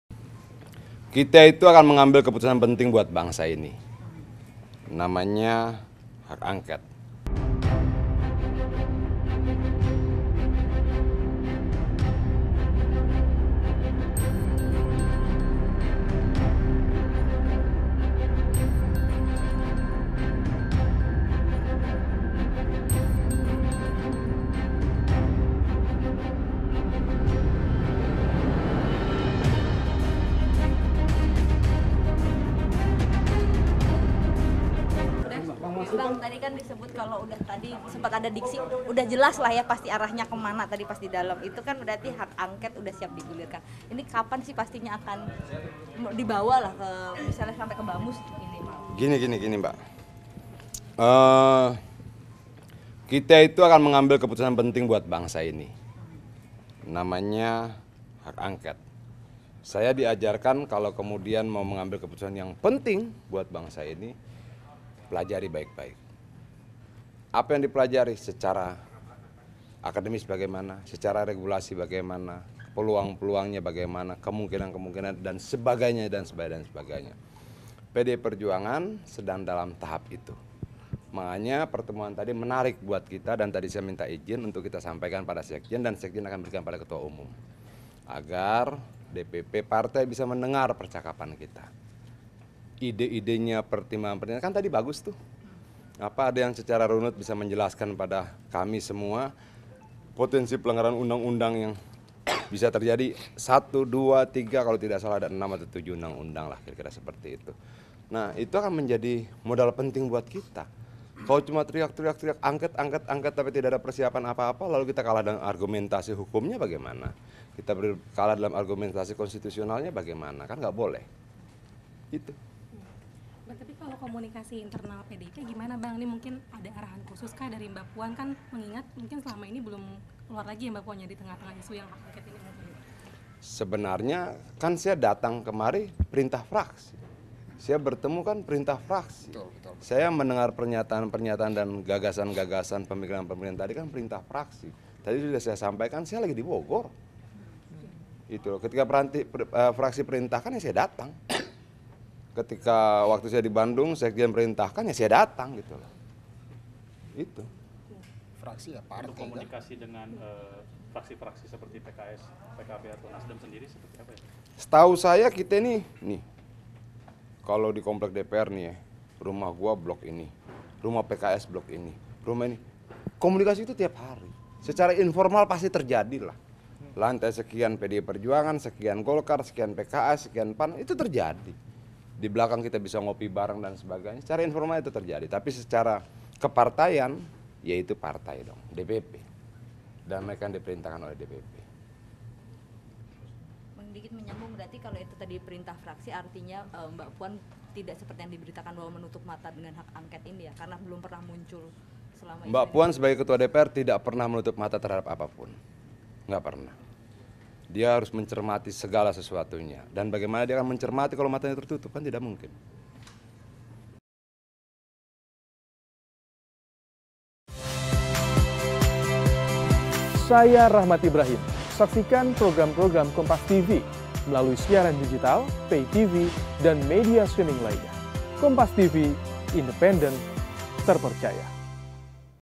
Adian Napitupulu Blak blakan soal Sikap Puan Terkait Hak Angket
JAKARTA, KOMPASTV  Politisi PDIP Adian Napitupulu sampaikan sikap Puan Maharani terhadap hak angket.

"Mbak Puan sebagai Ketua DPR tidak pernah menutup mata terhadap apa pun, nggak pernah. Dia harus mencermati segala sesuatunya," kata Adian, Selasa (19/3/2024).

Adian menjelaskan hak angket adalah keputusan penting buat bangsa untuk itu perlu ada persiapan berkas yang perlu dipelajari baik-baik.

"Apa yang dipelajari secara akademis bagaimana, secara regulasi bagaimana, peluang-peluangnya bagaimana, kemungkinan-kemungkinan dan sebagainya dan sebagainya. PDI perjuangan sedang dalam tahap itu," ucap Adian.

Baca Juga Ditanya Kapan Usulkan Hak Angket, Adian Napitupulu: Tinggal Tunggu Perintah Megawati di